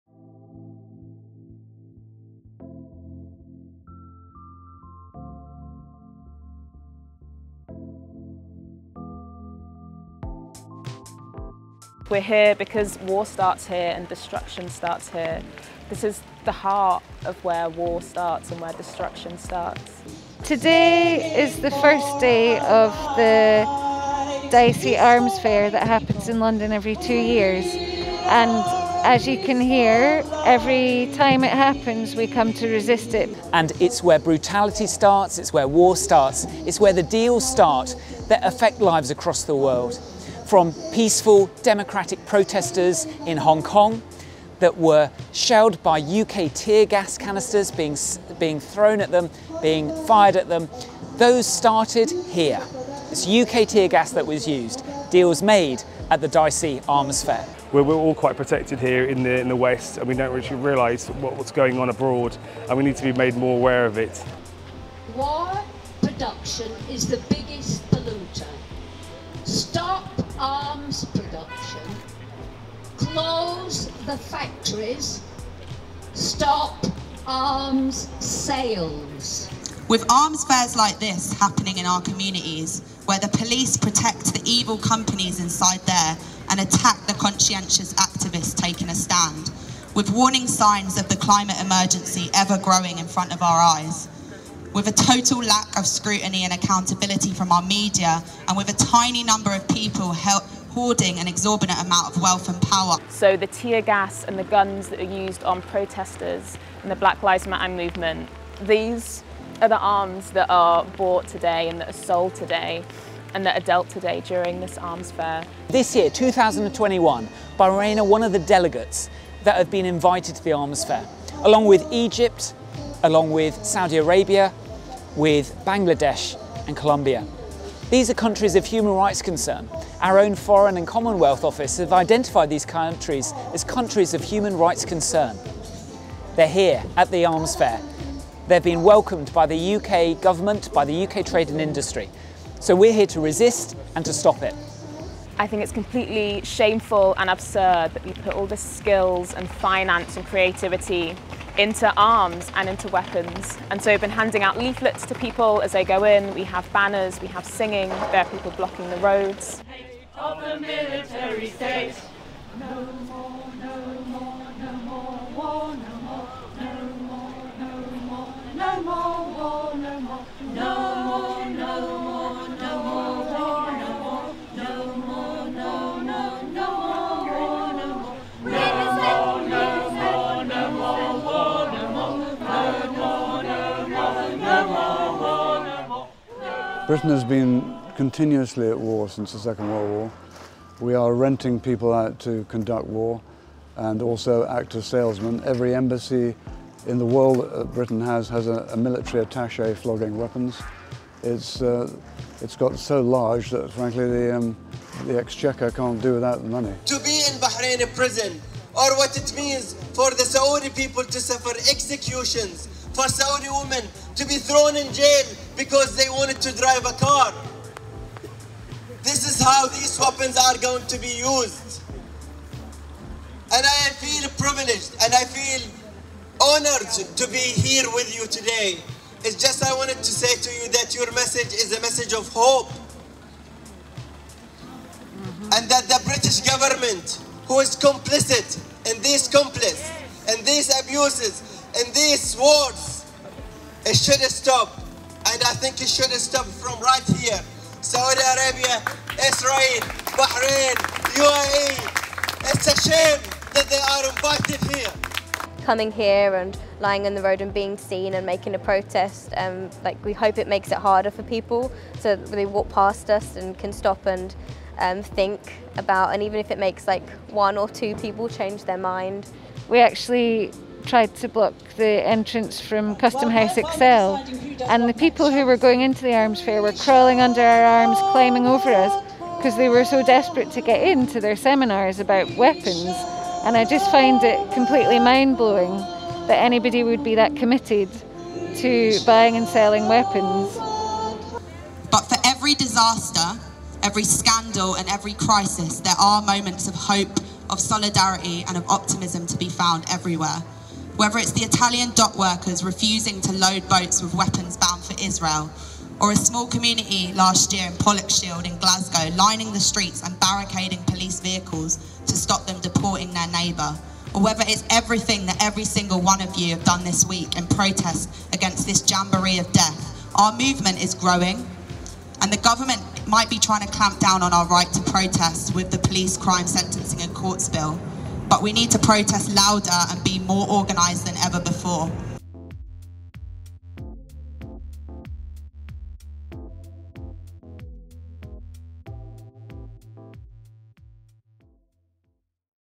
DSEI 2021 Highlights | The Struggle Continues | Video by Rainbow Collective
DSEI returned to London for another year in 2021, but as always, the Stop the Arms Fair Network and other comrades were there to send a strong message of resistance to the organisers and attendees.

Check out this highlights video by Rainbow Collective and learn more about how to join the movement!

Check out various speeches and talks across the two-weeks on the Stop the Arms Fair YouTube channel